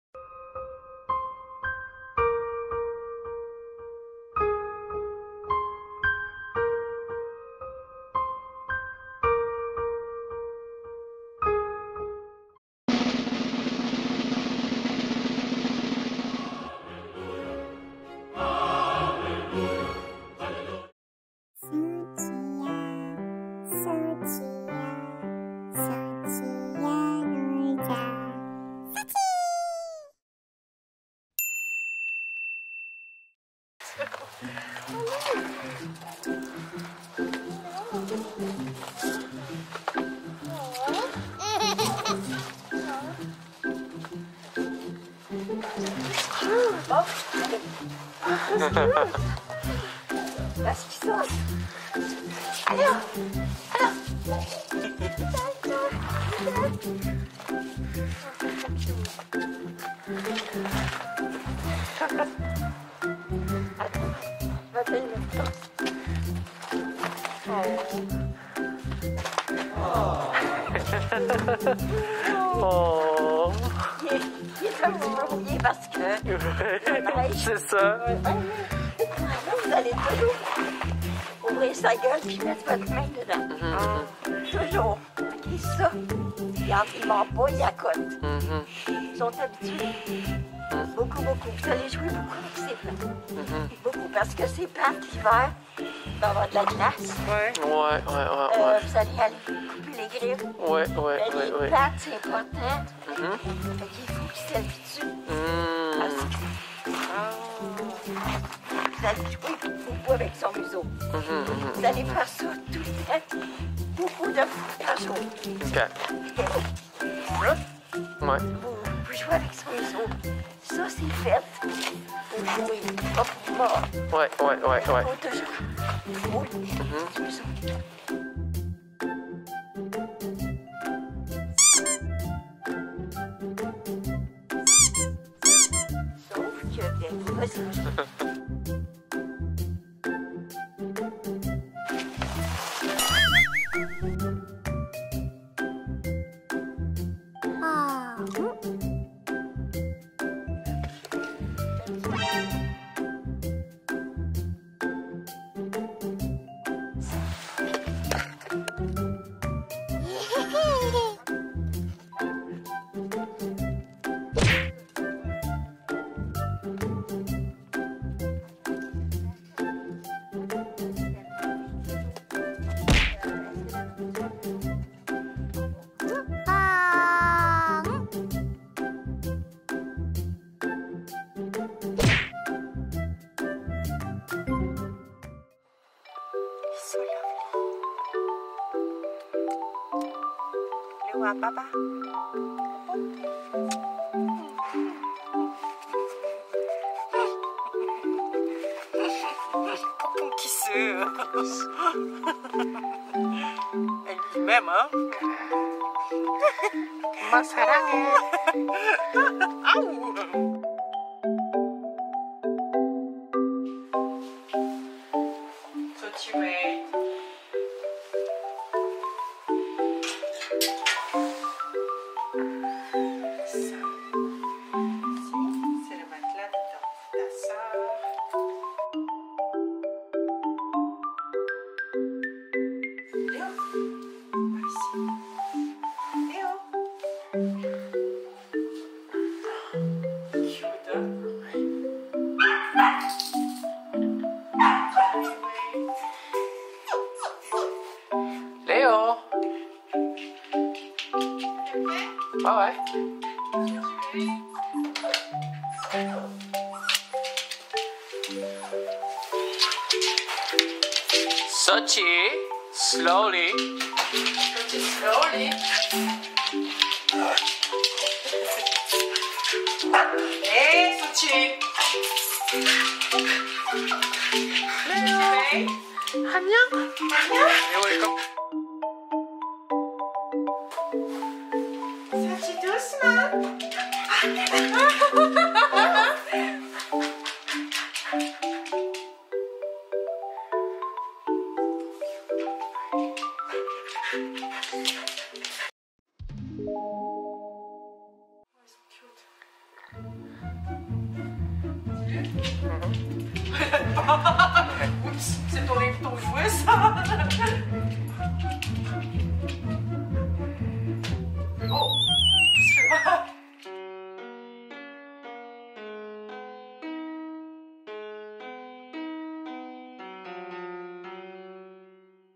When a Samoyed gets a Chow Chow puppy brother - Sochie meets Leo (Part I)
Hello my fellow Marshmallows!

I've been feeling quite lonely these days.

I've also always wanted a younger sibling. To my surprise, Mommy and Daddy went to pick up my new baby brother!

I'm so excited to be able to play with him!

안녕하세요^^

요즘 제가 집콕을 많이 해서 그런지 좀 외로움을 느껴요.
예전부터 동생이 생기길 바랬는데, 엄마 아빠가 제 맘을 읽고 남동생을 데려왔어요!
이제부터 같이 놀 수 있는 동생이 있어서 너무 좋아요 ㅎㅎ

또 만나요 :)